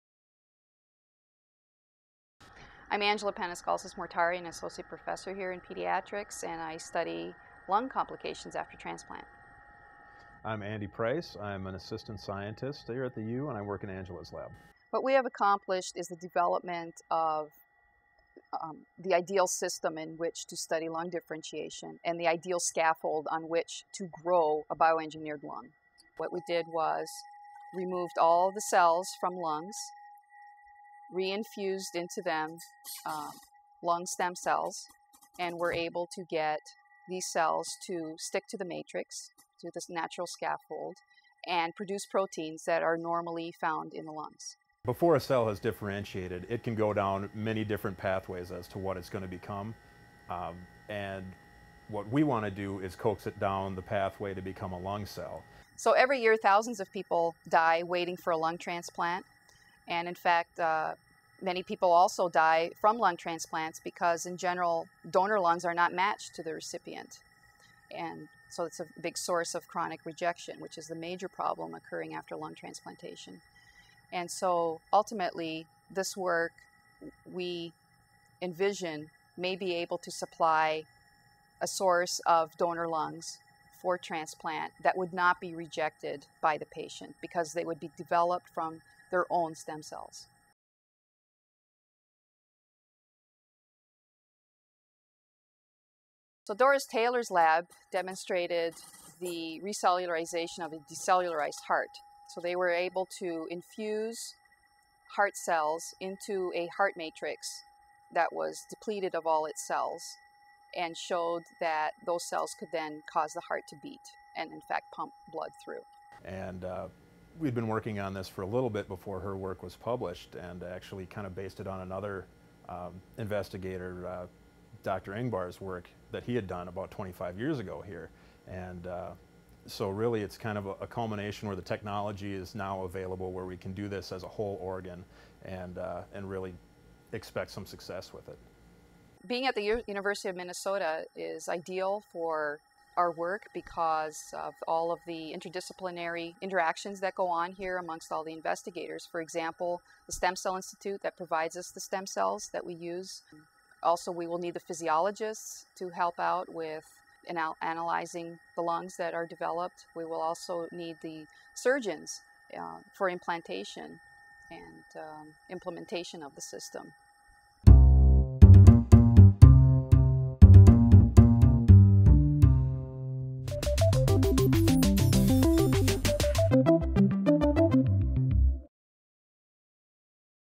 U of M scientists create breathing lungs in the lab
Scientists at the University of Minnesota's Masonic Cancer Center and Medical School have achieved another research first -- creating breathing lungs in the laboratory. This innovation comes two years after another group of University of Minnesota researchers used a similar technique to create a beating heart in the laboratory.